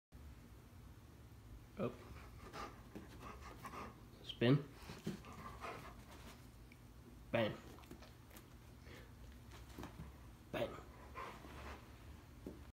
A few tricks greyhound/Husky breed
I got to do a few things this is my son Oakley

@dogs dog tricks @geryhounds